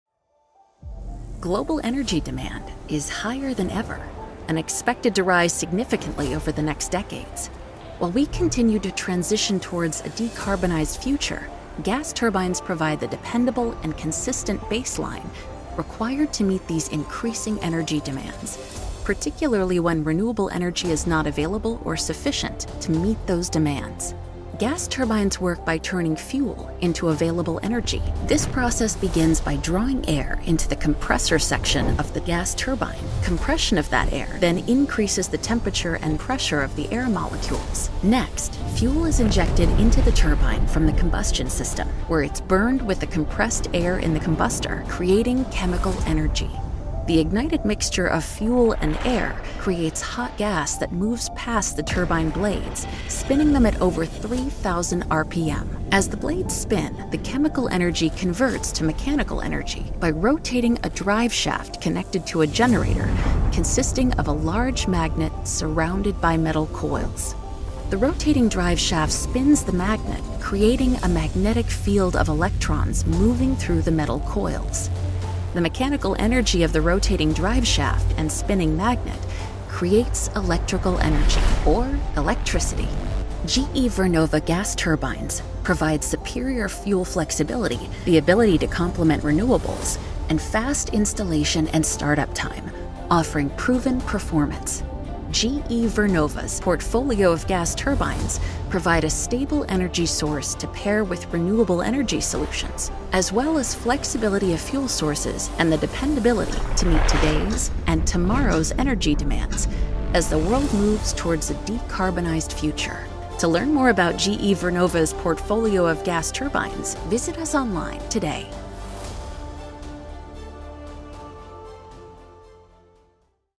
How a gas turbine works | GE Vernova
This video explains how a gas turbine, the heart of the power plant, produces an electric current that delivers power to our people. Put that in your power plant and spin it.